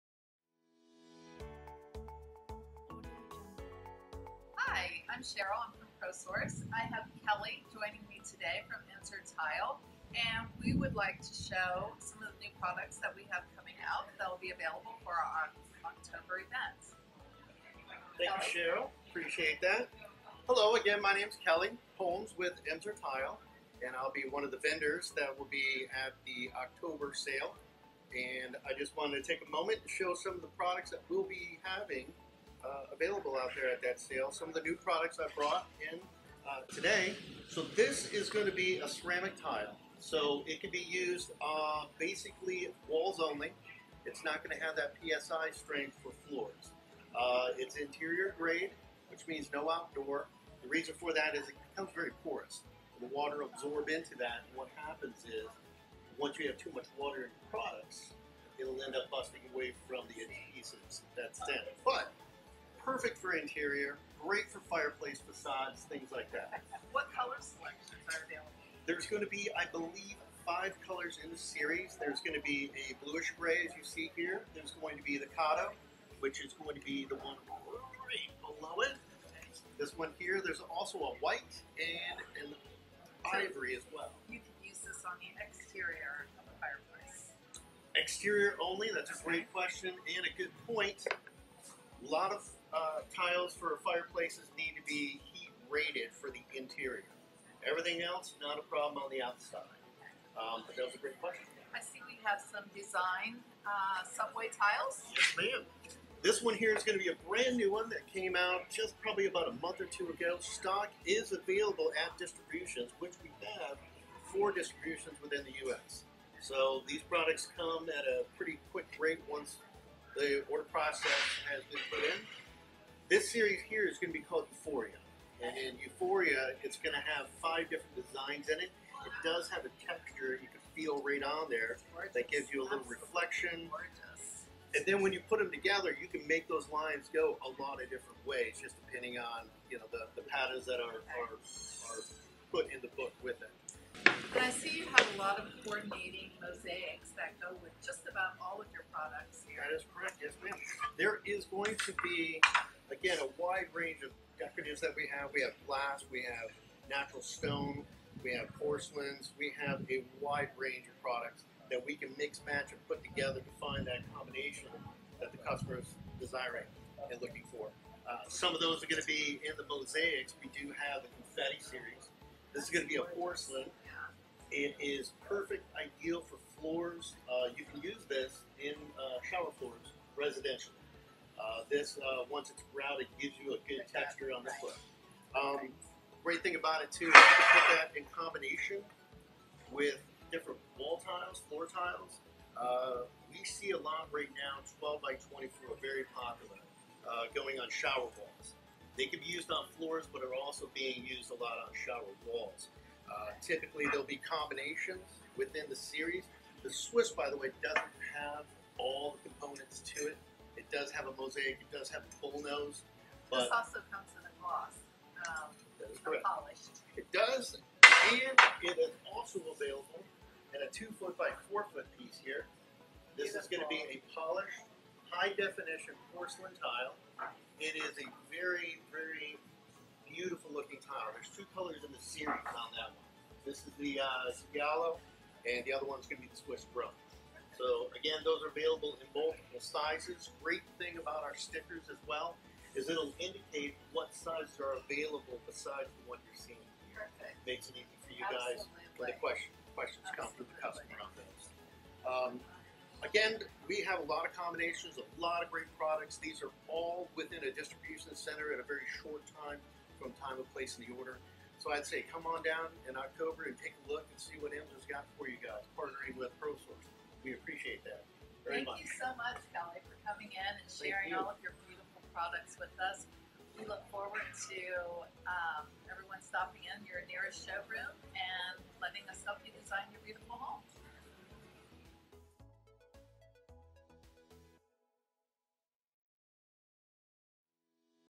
ProSource Orlando - Tile Options
Emser Tile will be partnering with us during our October Sales Event.  The most popular designs in all sizes, including large format tile and decorative tile.  Contact our Showroom for more information.